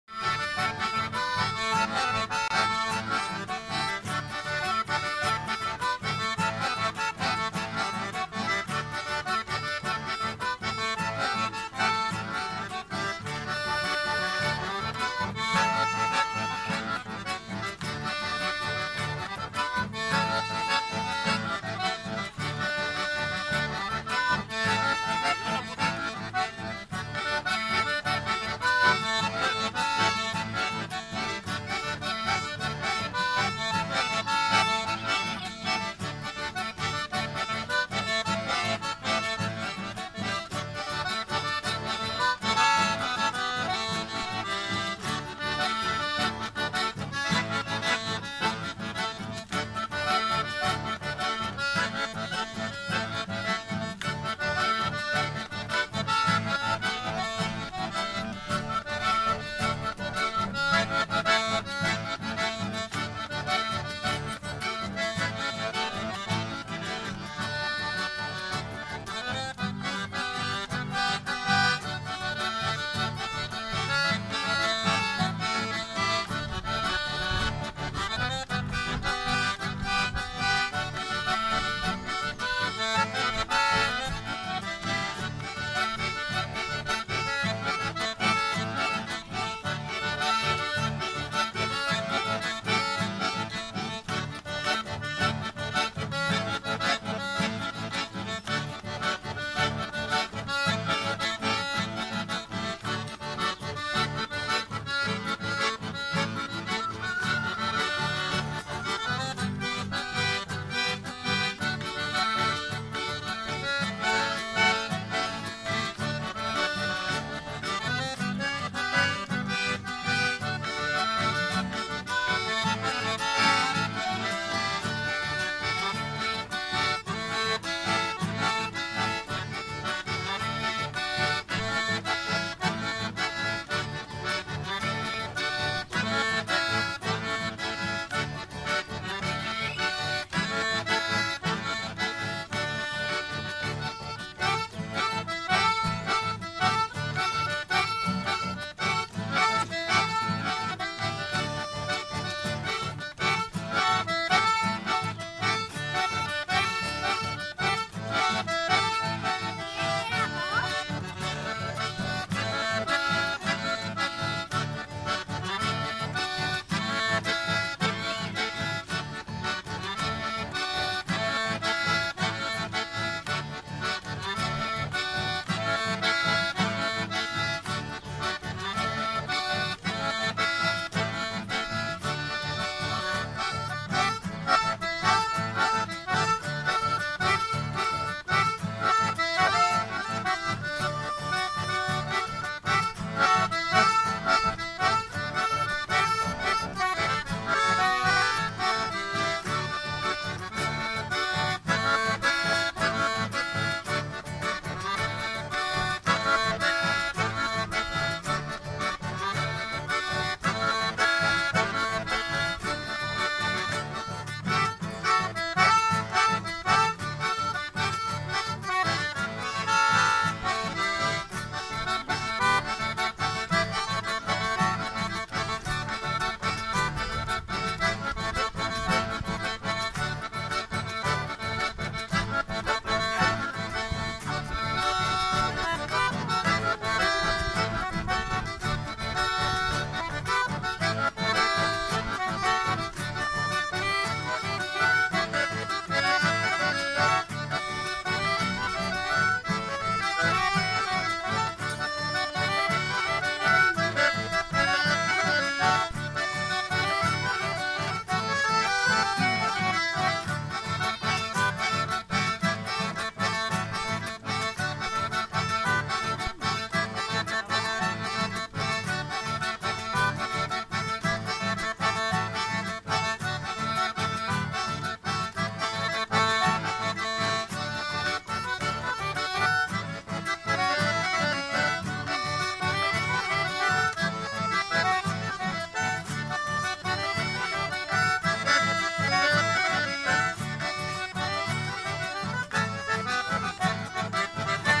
Chichin Villalba y Orlando martearena desde el espinillo Chaco
Músicos de nuestro querido norte chaqueño